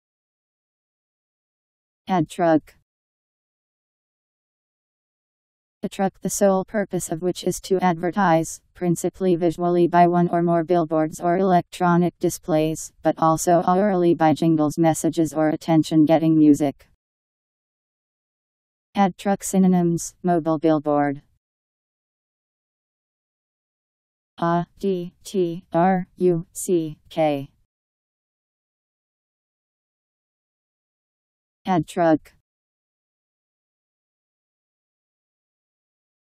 Ad truck Meaning
Video shows what ad truck means. A truck the sole purpose of which is to advertise, principally visually by one or more billboards or electronic displays, but also aurally by jingles messages or attention-getting music.. ad truck synonyms: mobile billboard.  Ad truck Meaning. How to pronounce, definition audio dictionary. How to say ad truck. Powered by MaryTTS, Wiktionary